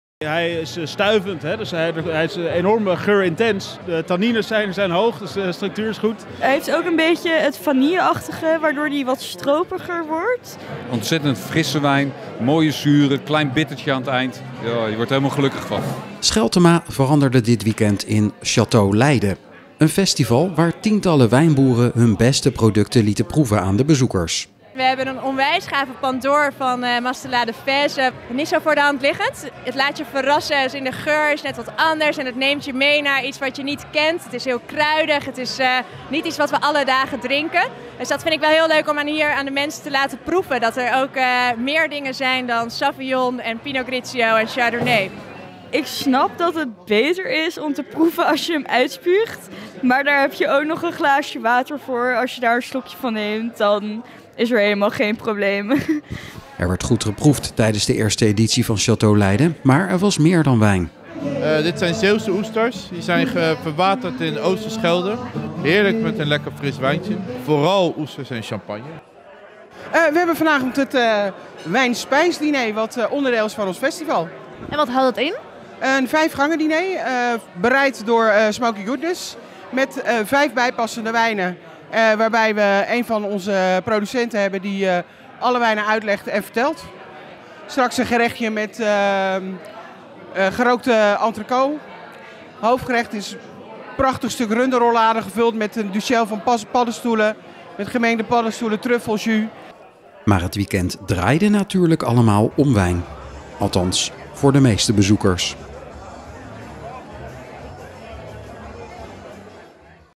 Chateau Leyden in Scheltema (23 oktober 2022)
Het hele weekend veranderde Scheltema in Chateau Leyden. Een festival waar tientallen wijnboeren hun beste producten lieten proeven aan de bezoekers.